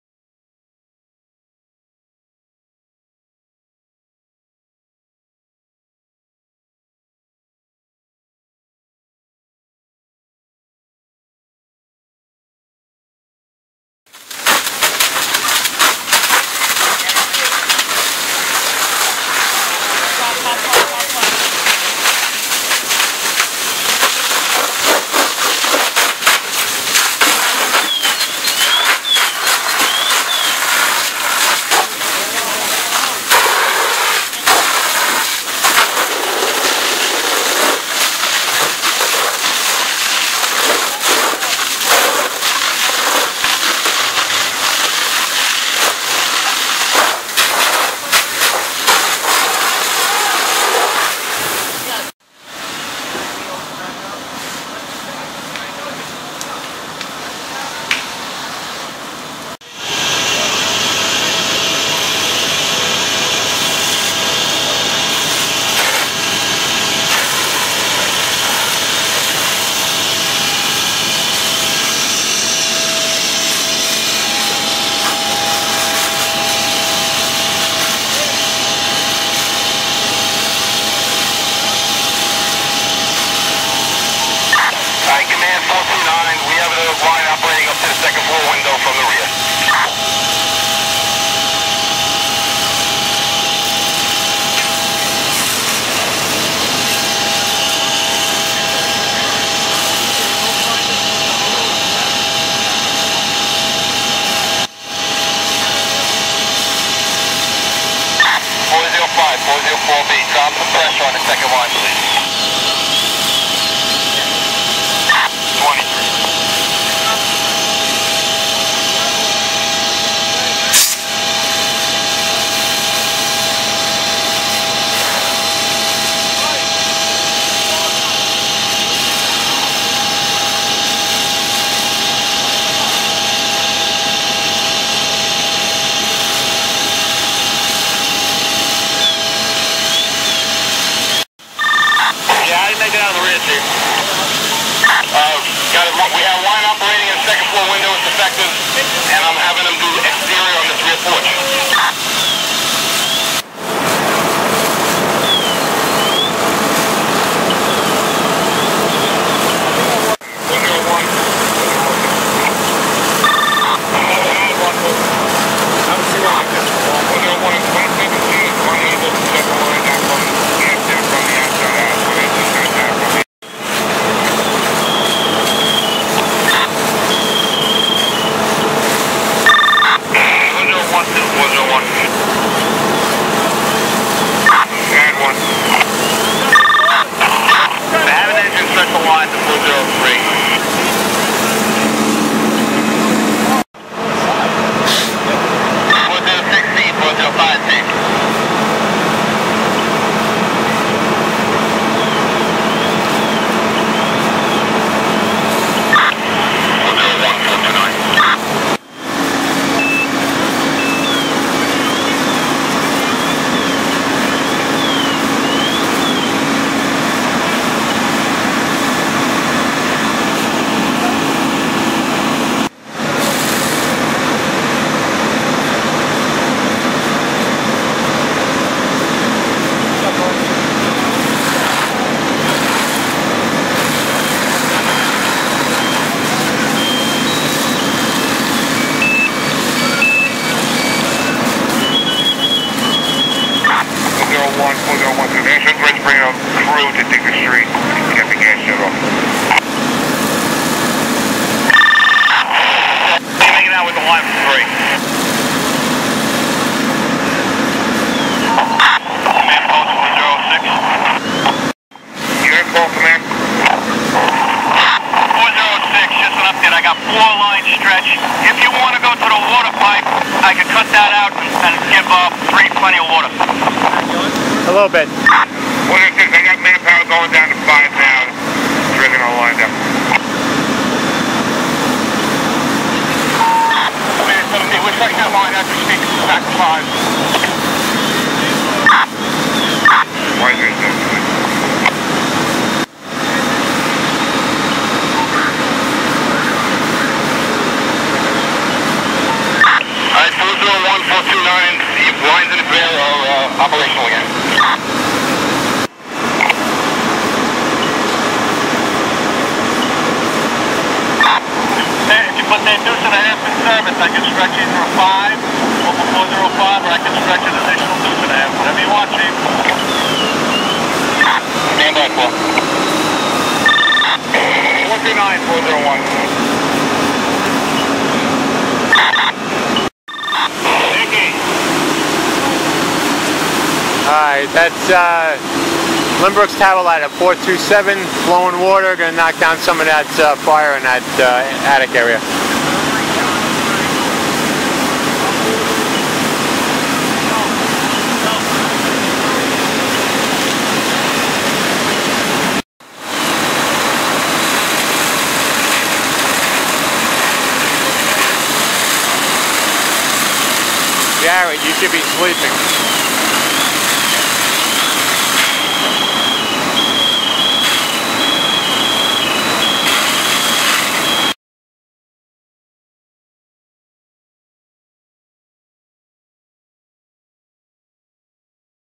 EAST ROCKAWAY 11-2-17 (BILLBENNETTPHOTO)
WORKING HOUSE FIRE 78 WAVERLY AVE EAST ROCKAWAY